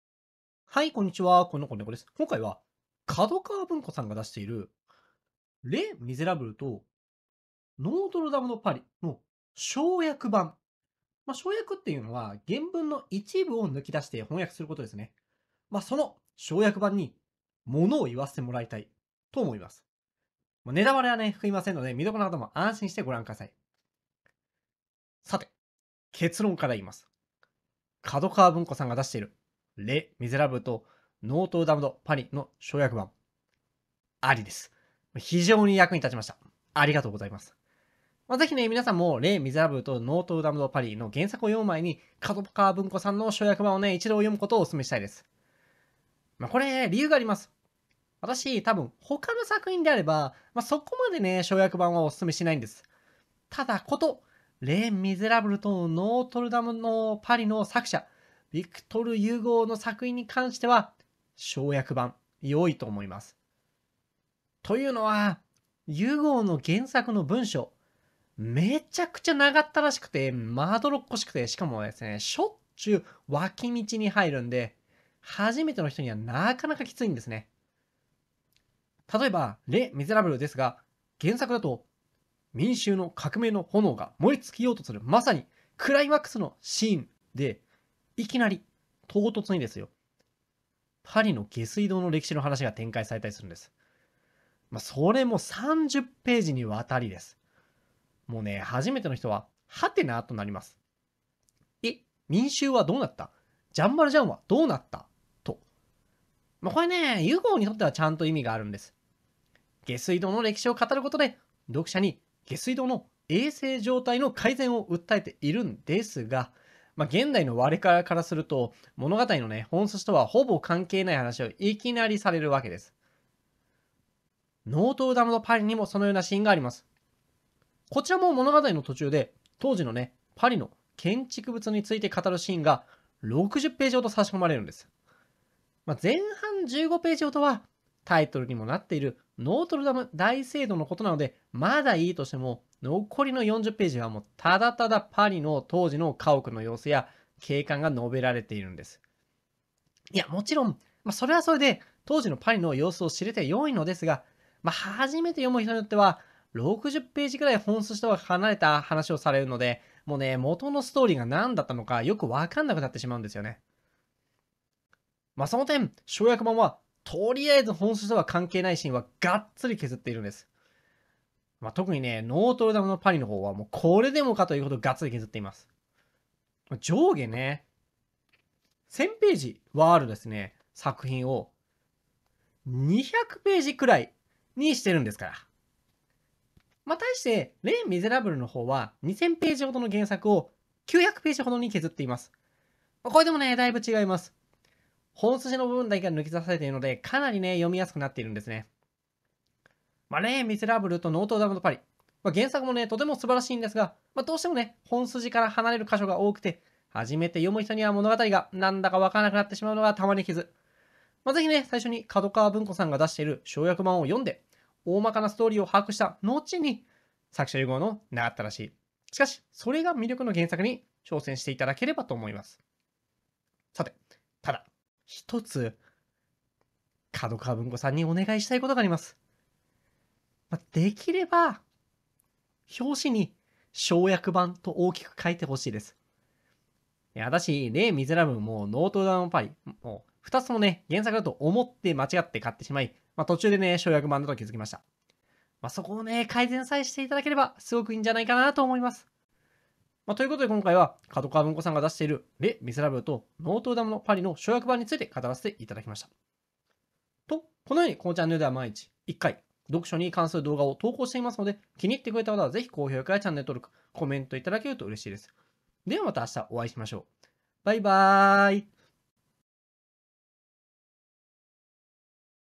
『レ・ミゼラブル』『ノートルダム・ド・パリ』の抄訳版について語ります
【紹介本】
『レ・ミゼラブル　抄訳版』
『ノートルダム・ド・パリ』